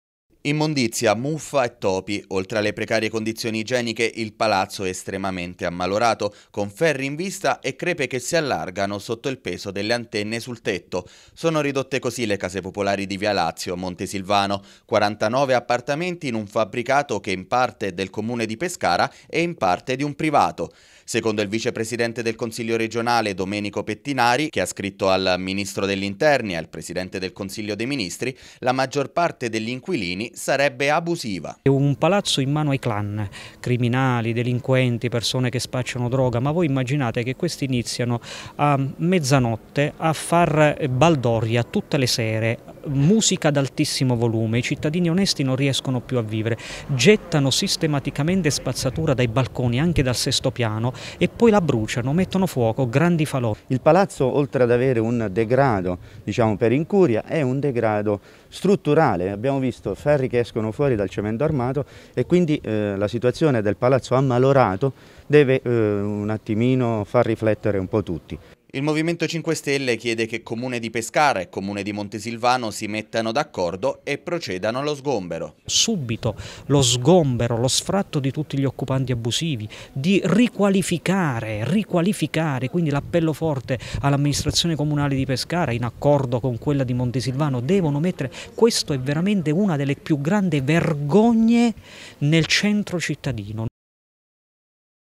Montesilvano. Il degrado delle palazzine di via Lazio
Sporcizia e problemi strutturali nel fabbricato di via Lazio a Montesilvano. Il consigliere regionale 5stelle Domenico Pettinari chiede subito lo sfratto degli inquilini abusivi.